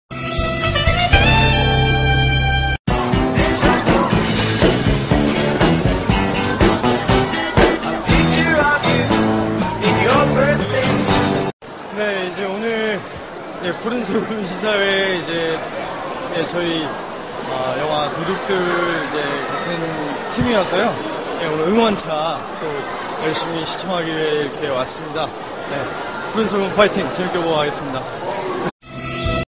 [20110823 News] Kim Soo Hyun at the "Blue Salt" movie VIP Premiere
Credit: sstv, soohyundreamer